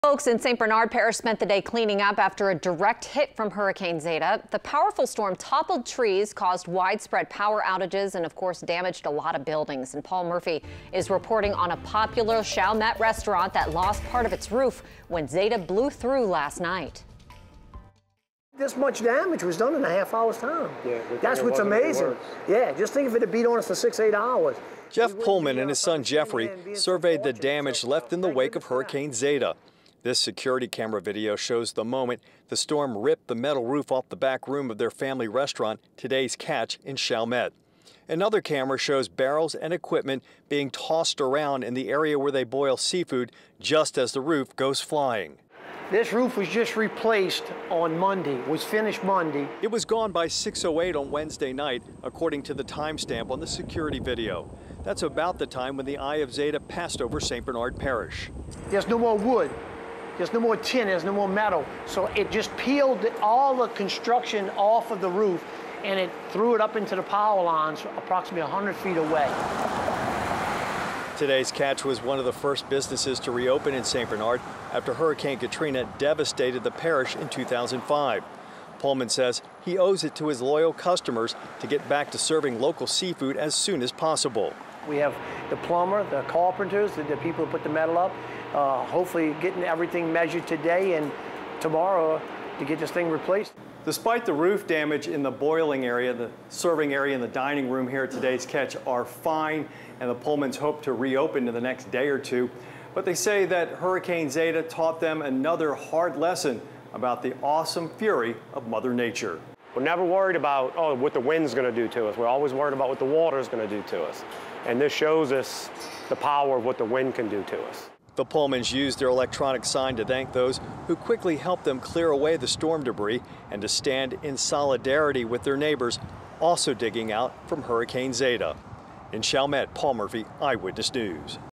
Zeta Aftermath: St. Bernard Parish
Chalmette's own Today’s Ketch got caught up in the path of Hurricane Zeta but is not going to let that keep them out of business.